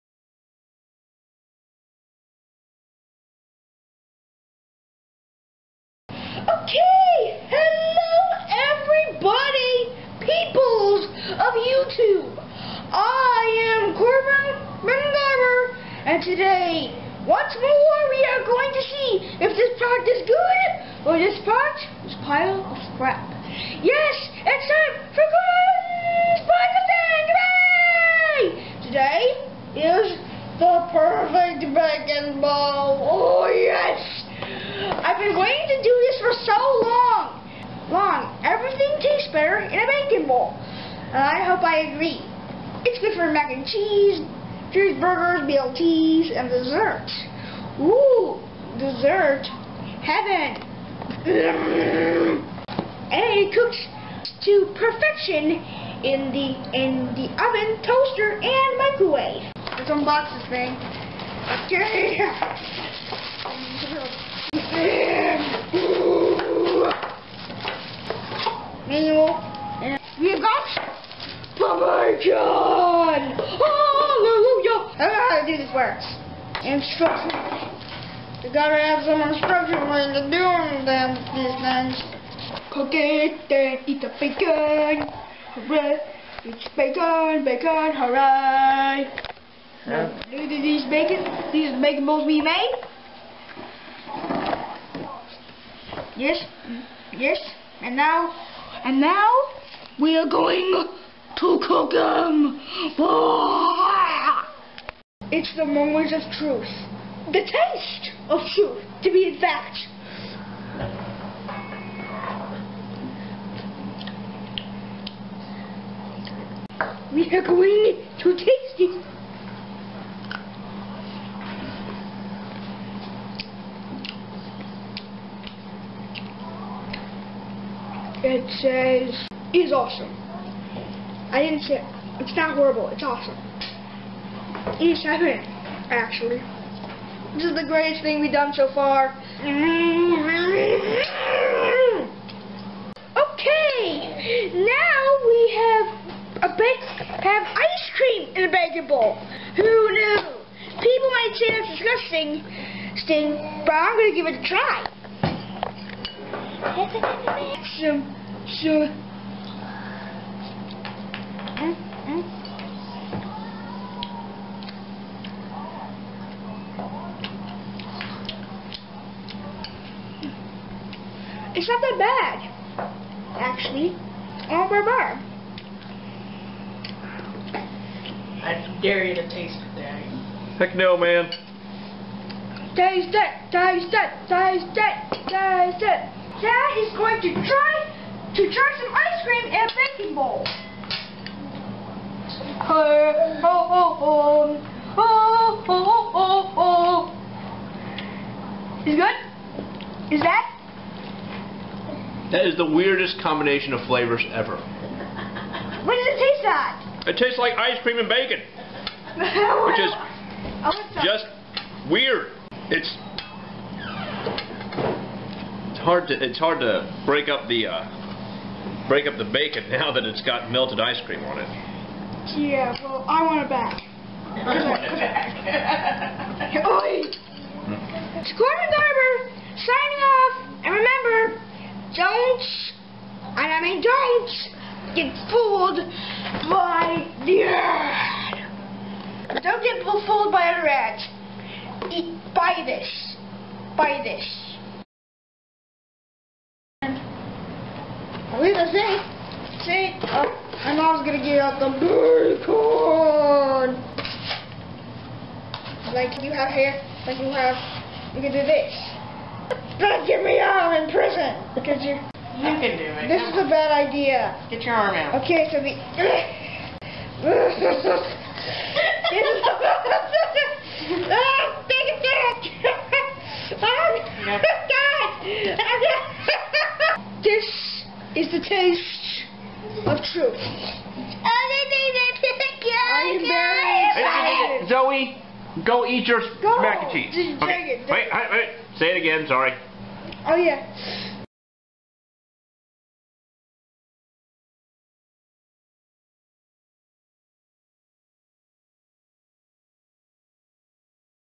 Corbin's Product Testing 3: The Perfect Bacon Bowl review
Corbin has been dying to do this product.  He loves bacon!!  Will his love of bacon get the Perfect Bacon Bowl a good review?